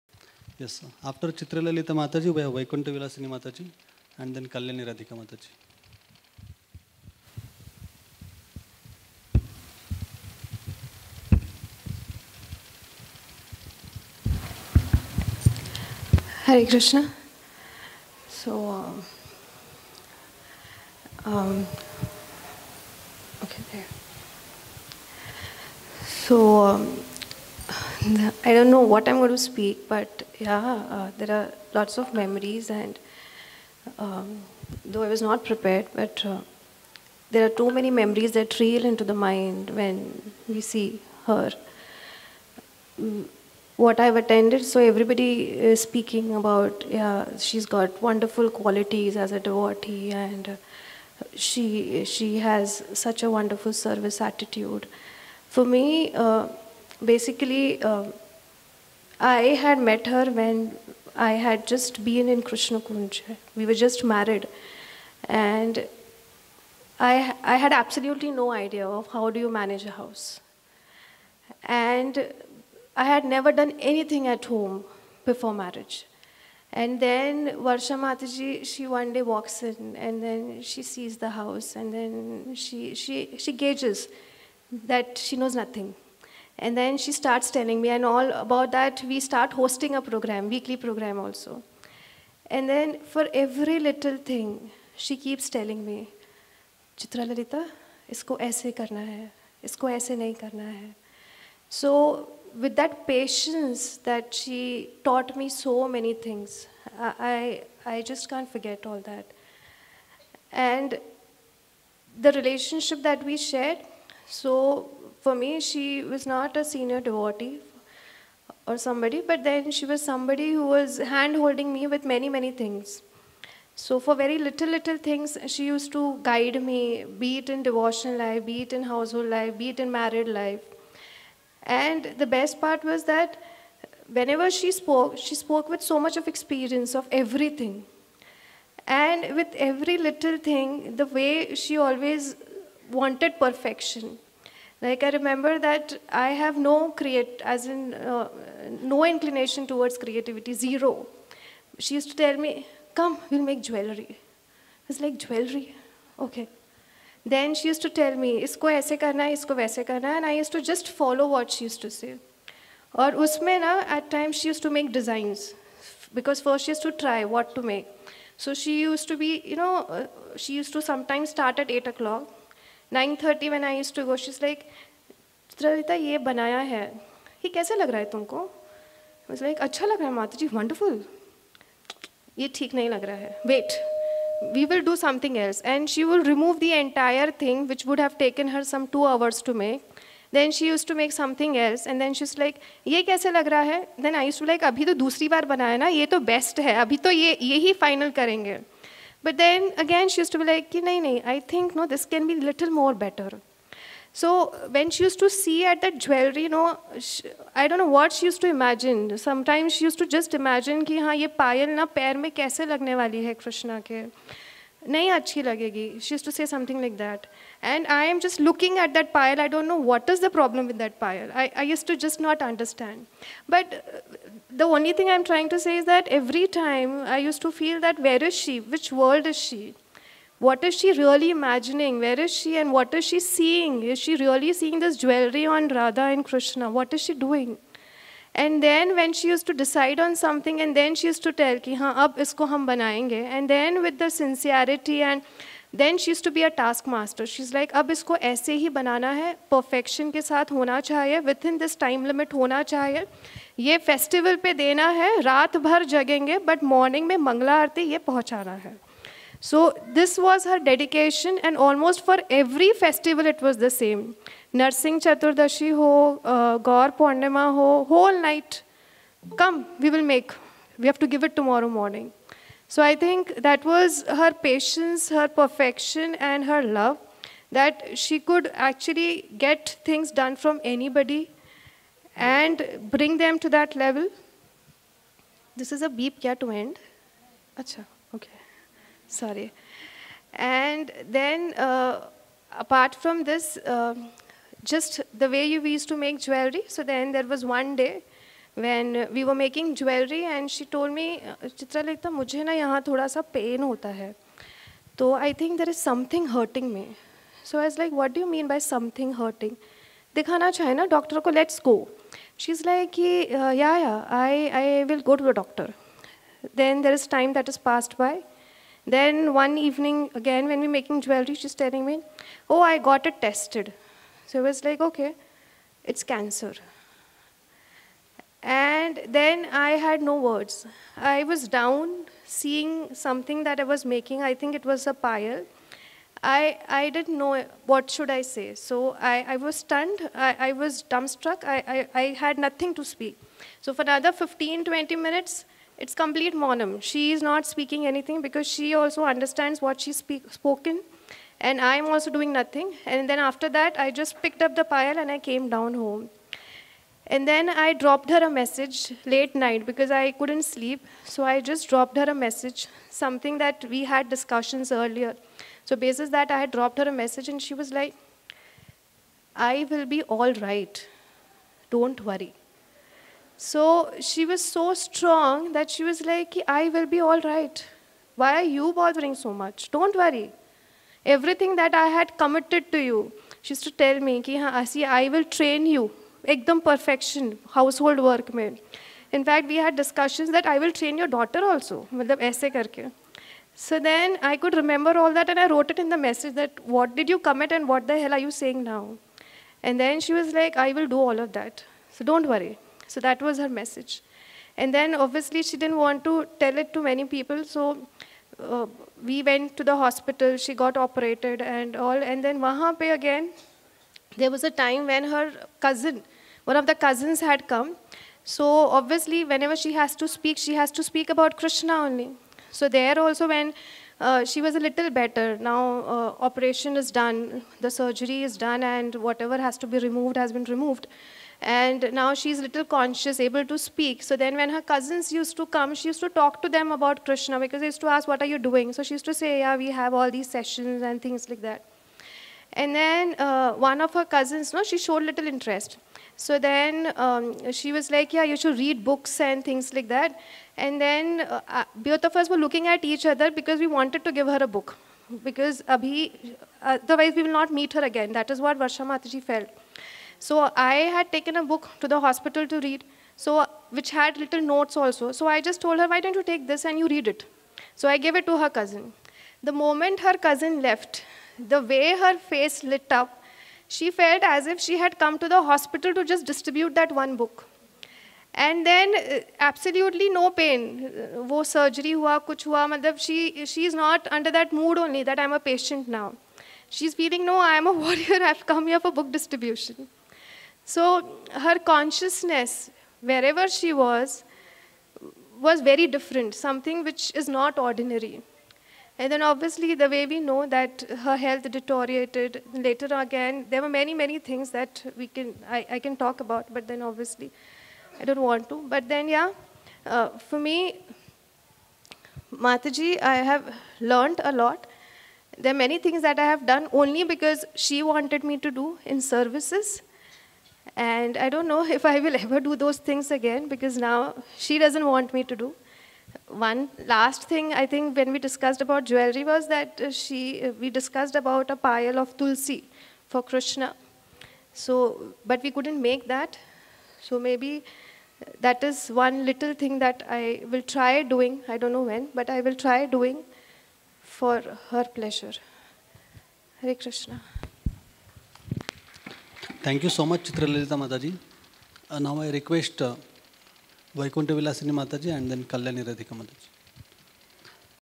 Part - 6/19 Vrishabhanu Nandini Mataji Memorial Program.
This is Memorial Program of Her Grace Vrishabhanu Nandini Devi Dasi, who left her body gloriously in presence of devotees chanting. She has lived her life completely dedicated to Krishna Consciousness and ISKCON. May Lord Krishna and Srimati Radharani bless her with higher destination in her journey of Krishna consciousness.

There was glitch in the audio connection while recording the program. We could recover only Video file. So you may find not so clear but the talk can be heard. Please excuse us for that _/\_
Program Organized by Tulsi Das and his spiritual counselee group members
Video Shooting Credits: Nathhari Das
Audio Sync: Ajit Kumar Tutika